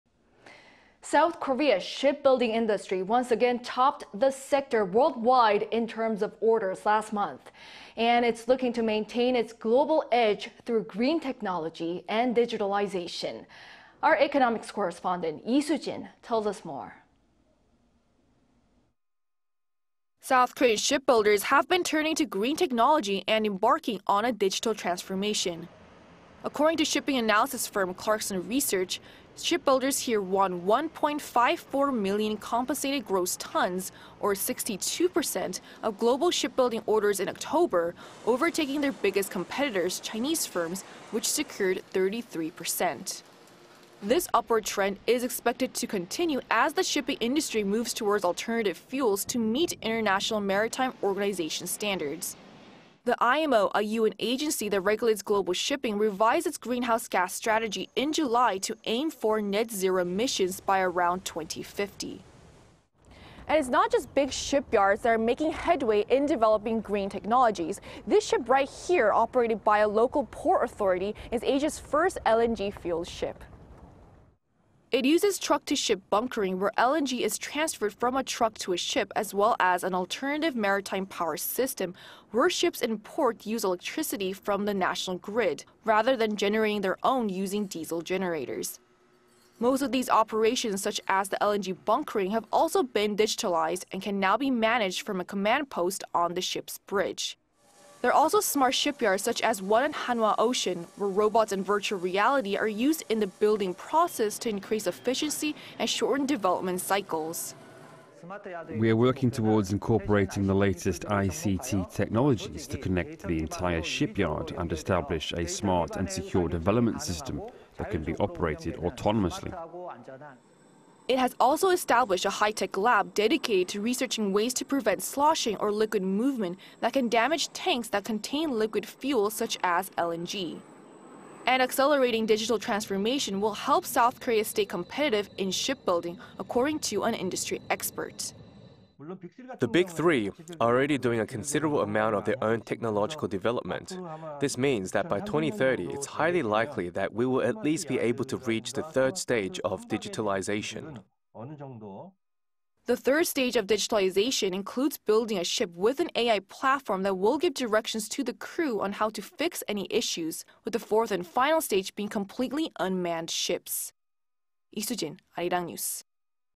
S. Korea's shipbuilding industry uses green tech, digitalization to stay on top in global orders
韓조선업, 친환경‧스마트 기술력으로 세계 선박 수주량 1위 달성 앞으로의 과제는?

South Korea's shipbuilding industry once again topped the sector worldwide in terms of orders last month.
And it's looking to maintain its global edge through green technology and digitalization.
Our economics correspondent Lee Soo-jin tells us more.

South Korean shipbuilders have been turning to green technology and embarking on a digital transformation.
According to shipping analysis firm Clarkson Research,
shipbuilders here won 1-point-54 million compensated gross tons, or  62 percent, of global shipbuilding orders in October, overtaking their biggest competitors, Chinese firms, which secured  33 percent.

This upward trend is expected to continue as the shipping industry moves toward alternative fuels to meet International Maritime Organization standards.

The IMO, a UN agency that regulates global shipping, revised its greenhouse gas strategy in July to aim for net-zero emissions by around 2050.

“And it's not just big shipyards that are making headway in developing green technologies. This ship, which is operated by a local port authority, is Asia’s first LNG-fueled ship.”

It uses truck-to-ship bunkering, where LNG is transferred from a truck to a ship, as well as an alternative maritime power system where ships in port use electricity from the national grid, rather than generating their own using diesel generators.
Most of these operations such as the LNG bunkering have also been digitalized and can now be managed from a command post on the ship's bridge.
There are also smart shipyards such as one at Hanwha Ocean, where robots and virtual reality are used in the building process to increase efficiency and shorten development cycles.

"We're working towards incorporating the latest ICT technologies to connect the entire shipyard and establish a smart and secure development system that can be operated autonomously."

It has also established a high-tech lab dedicated to researching ways to prevent sloshing, or liquid movement, that can damage tanks that contain liquid fuel such as LNG.

And accelerating digital transformation will help South Korea stay competitive in shipbuilding, according to an industry expert.

"The Big Three are already doing a considerable amount of their own technological development. This means that by 2030 it’s highly likely that we will at least be able to reach the third stage of digitalization."

The third stage of digitalization includes building a ship with an AI platform that will give directions to the crew on how to fix any issues, with the fourth and final stage being completely unmanned ships.

Lee Soo-jin, Arirang News.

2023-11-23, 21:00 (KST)